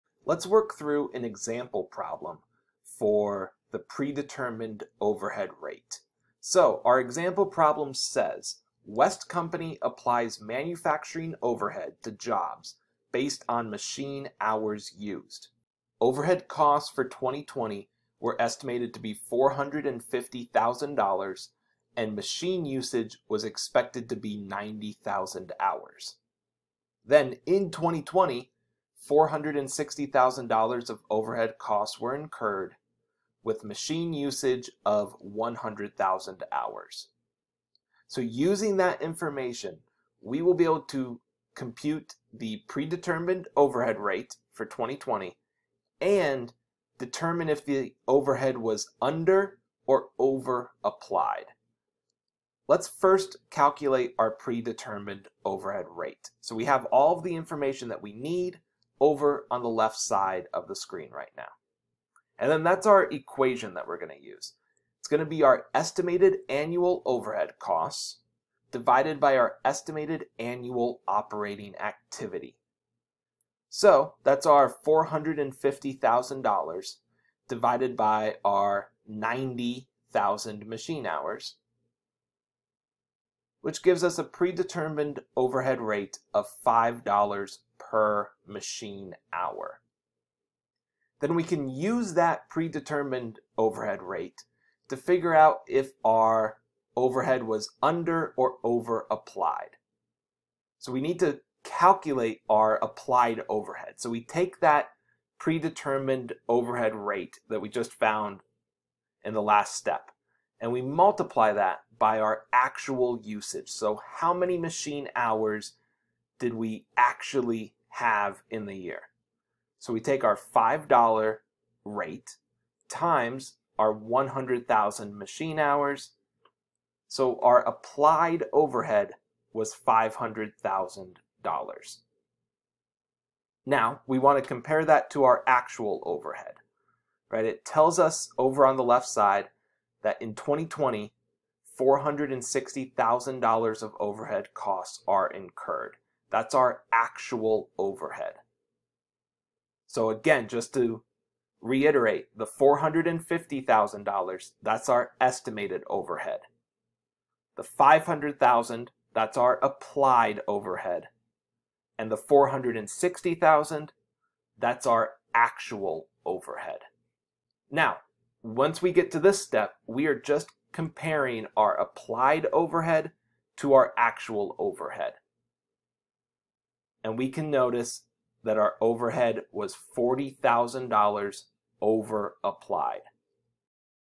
Predetermined Overhead Rate
Reviewing how to find the predetermined overhead rate, and determine if the overhead was under-applied or over-applied.